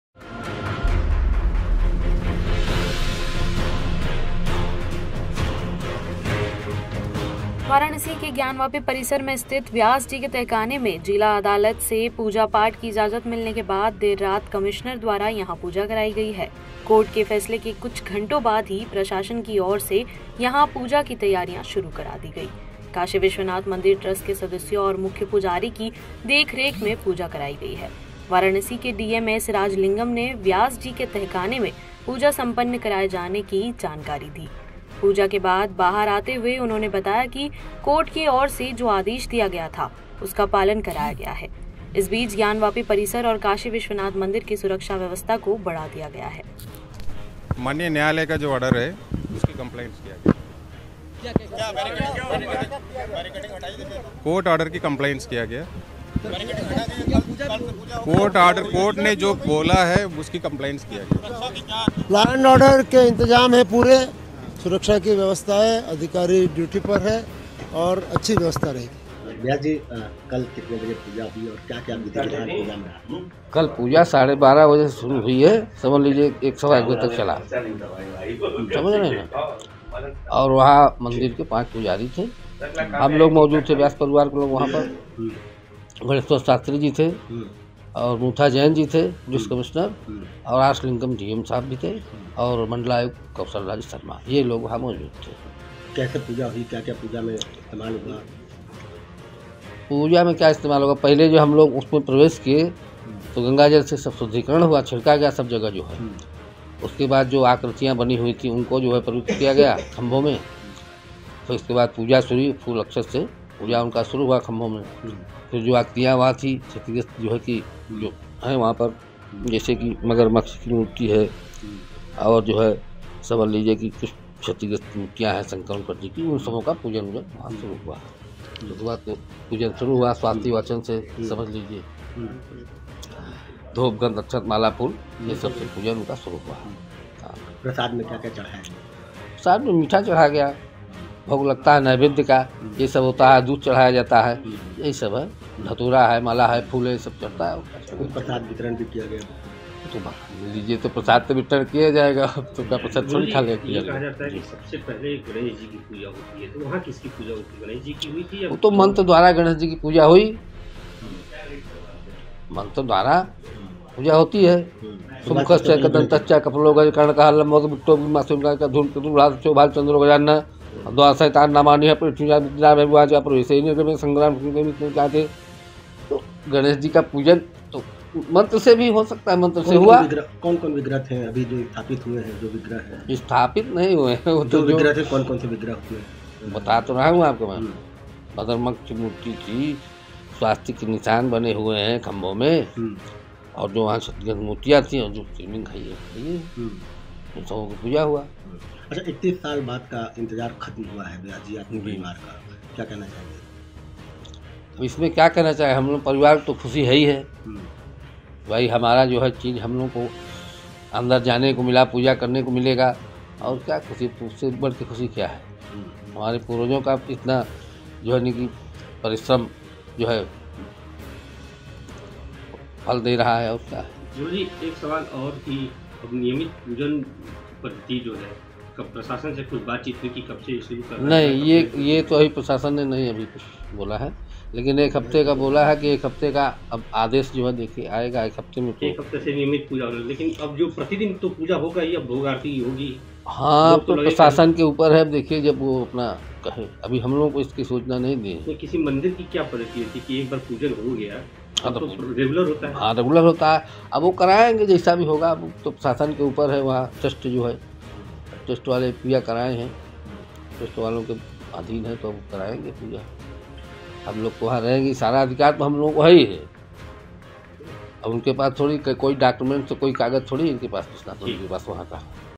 Gyanvapi Latest News : ज्ञानवापी में गंगाजल छिड़कते ही हुआ बड़ा चमत्कार | Gyanvapi Masjid Case | N18
After getting permission from the district court to perform the puja in the basement of Shri Vyas ji in the Gyanvapi complex of Varanasi, the puja was conducted here by the Commissioner late at night. A few hours after the court's decision, the administration started preparations for the puja here. The puja has been conducted under the supervision of the members of the Kashi Vishwanath Temple Trust and the chief priest. Varanasi DM S Rajalingam informed about the puja being conducted in the basement of Vyas ji.

वाराणसी के ज्ञानवापी परिसर में स्थिति व्यास जी के तहखाने में जिला अदालत से पूजा पाठ की इजाज़त मिलने के बाद देर रात कमिश्नर द्वारा यहां पूजा कराई गई है. कोर्ट के फैसले के कुछ घटों बाद ही प्रशासन की ओर से यहाँ पूजा की तैयारियां शुरू करा दी गईं. काशी विश्वनाथ मंदिर ट्रस्ट के सदस्यों और मुख्य पुजारी की देखरेख में पूजा कराई गई है. वाराणसी के डीएम एस राजलिंगम ने व्यास जी के तहख़ाने में पूजा संपन्न कराए जाने की जानकारी दी.